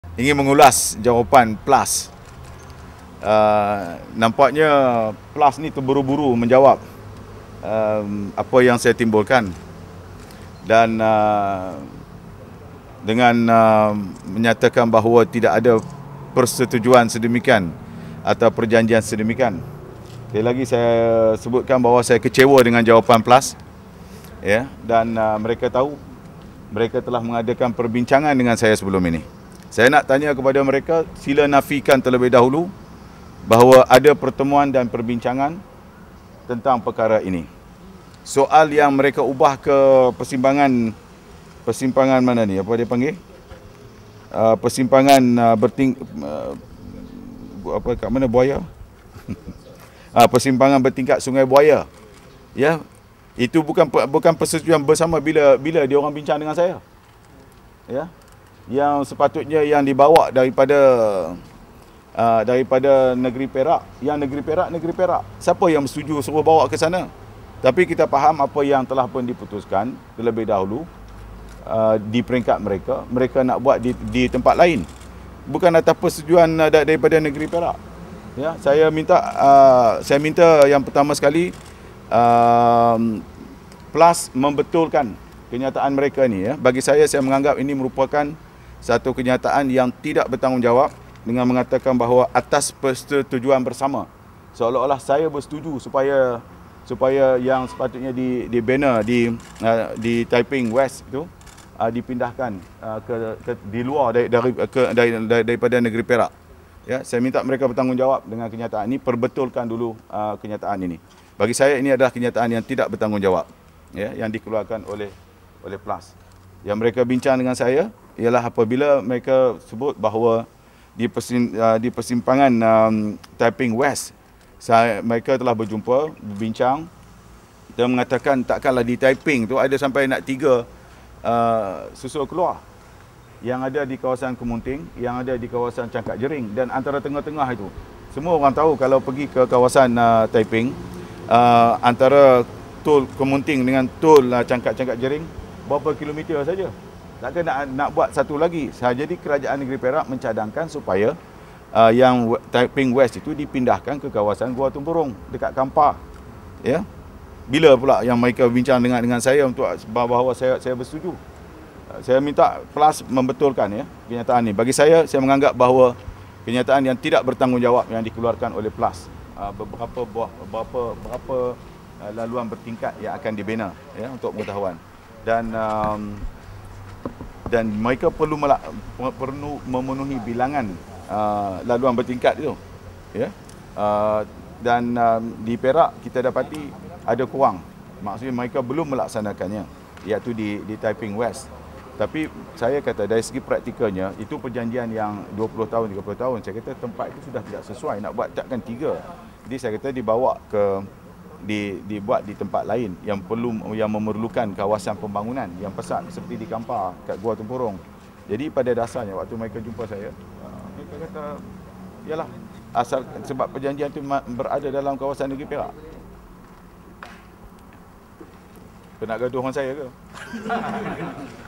PLUS Keluarkan Kenyataan Tidak Bertanggungjawab:- DSD Dr Zambry Abdul Kadir
11/04/2014 -- PLUS Keluarkan Kenyataan Tidak Bertanggungjawab:- DSD  Dr Zambry Abdul Kadir

Kerajaan Perak menyelar tindakan PLUS yang mengeluarkan kenyataan balas tidak bertanggungjawab dalam isu naiktaraf lebuh raya.

Menteri Besar, Datuk Seri Dr Zambry Abdul Kadir berkata, pihak Plus bertindak secara terburu-buru dan mengeluarkan kenyataan tidak berasas dalam isu itu.

"Saya ingin mengulas mengenai kenyataan Plus, nampaknya Plus ini mengeluarkan kenyataan yang terburu-buru dalammenjawab apa yang timbulkan.

"Mereka menyatakan bahawa tidak ada persetujuan sedemikian.

"Sekali lagi saya sebutkan bahawa saya kecewa dengan jawapan Plus.

"Mereka tahu bahawa mereka telah mengadakan perbincangan dengan saya sebelum ini.

Dalam pada itu, Zambry menegaskan bahawa pihak Plus perlu menafikan kenyataan wujudnya pertemuan atau perbincangan bersama pihaknya terlebih dahulu.

Katanya, kerajaan negeri tidak akan membuat sebarang persetujuan sekiranya ia melibatkan kawasan luar daro negeri itu.

"Saya nak tanya kepada mereka, sila nafikan terlebih dahulu bahawa ada pertemuan dan perbincangan tentang perkara ini.

"Soal mereka ubah yang persimpangan laluan bertingkat Sungai Buaya, itu bukan persetujuan bersama bila mereka bincang dengan saya.

" Perkara yang sepatutnya yang dibawa daripada negeri Perak, siapa yang bersetuju ia dibawa ke sana ke kawasan lain, ia bukan atas persetujuan kerajaan Perak.

"Saya mahu Plus membetulkan kenyataan mereka, bagi kita ia satu kenyataan yang tidak bertanggungjawab dengan menyatakan bahawa atas persetujuan bersama.

Zambry menambah, tindakan itu menunjukkan seolah-olah kerajaan Perak telah bersetuju bahawa laluan yang sepatutnya dibina di Taiping itu dipindahkan di luar daripada negeri Perak.

"Saya mahu mereka bertanggungjawab dengan kenyataan mereka.

"Mereka bincang dengan saya ialah mereka sebut bahawa di persimpangan Taiping West untuk dibina susur keluar.

"Tetapi takkanlah di Taiping sahaja ada sampai 3 susur keluar.

"Yang kerajaan negeri mahu adalah susur keluar itu dipindahkan kepada Gua Tempurung.

"Itu yang mereka bincang dengan saya dan saya bersetuju," katanya lagi.